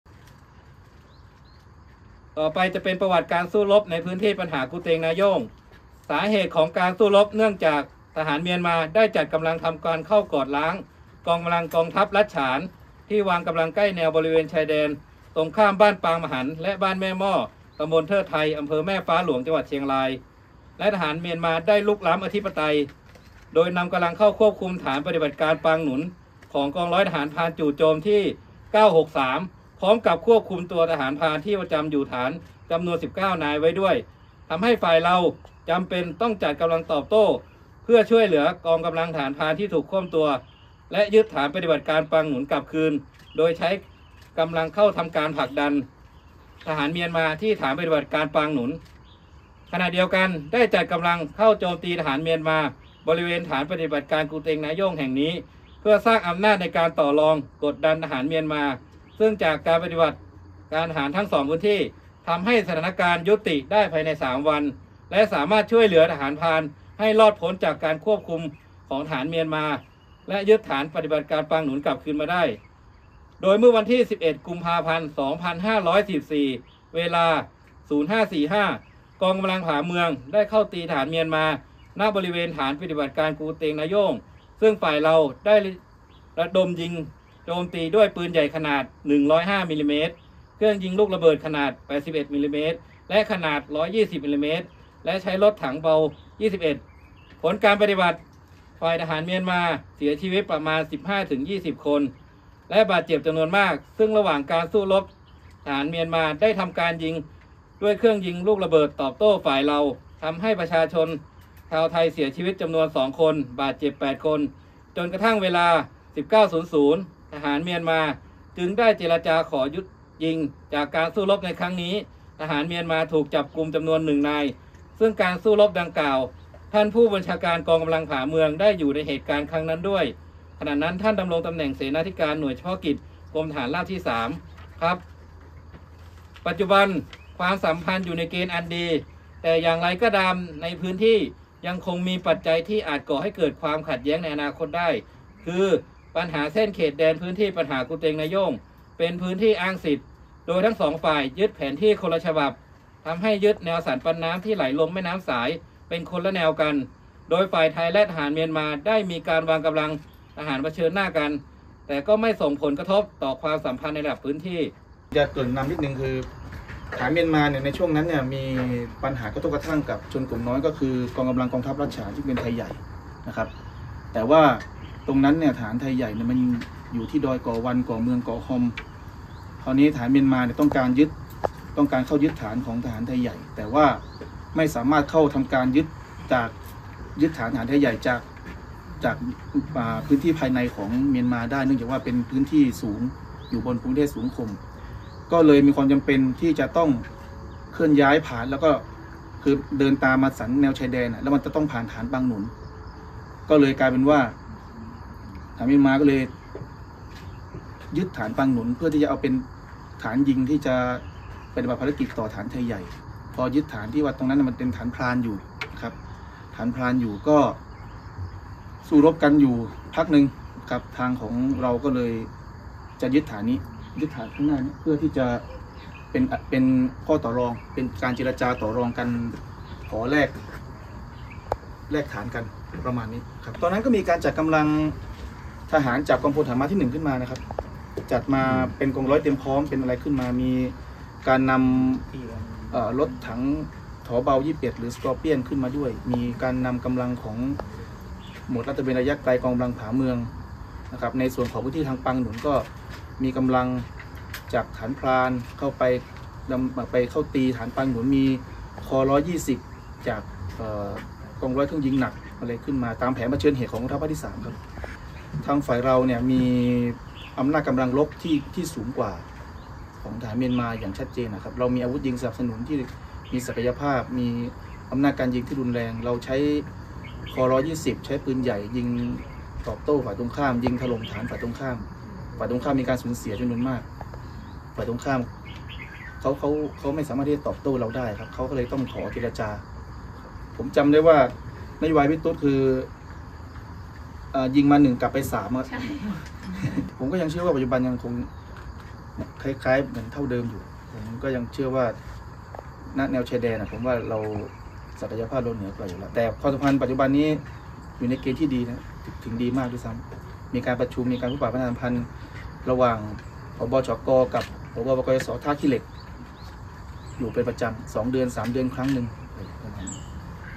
TAF On Tour 7 - ย้อนอดีตสงครามไทย-พม่าที่กู่เต็งนาโย่ง ปี 2544
การบรรยายสรุปของกำลังพลที่ฐานปฏิบัติการ กู่เต็งนาโย่ง เพื่อย้อนอดีตไปในปี 2544 เมื่อครั้งที่กองทัพ พม่า บุกฐานทหารไทยเพื่อใช้พื้นที่เข้าทำการรบกับชนกลุ่มน้อย จนกองทัพภาคที่ 3 กองทัพบก ต้องเปิดปฏิบัติการยึดฐานกลับคืน ซึ่งสร้างความสูญเสียให้กับฝั่ง เมียนมาร์ ค่อนข้างมาก ถือเป็นประวัติศาสตร์การรบหน้าหนึ่งของกองทัพไทย ซึ่งในปัจจุบันทั้งสองฝ่ายมีความสัมพันธ์อันดีต่อกัน

เยี่ยมชม TAF ได้ที่